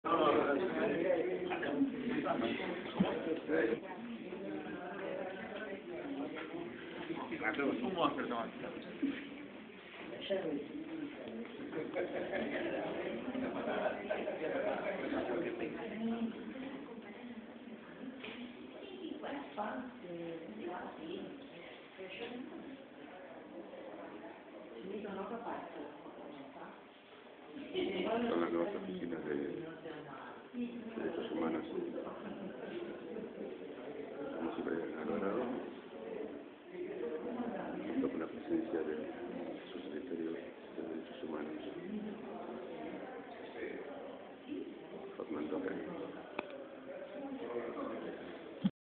290411-1126 Inauguraciòn nueva oficina Dirección de Derechos Humanos MGA.3gp
Miramar, Hoy pasadas las 11,30 hs. se inauguró la nueva oficina de la Dirección de Derechos Humanos, ubicada en el subsuelo del Palacio Municipal.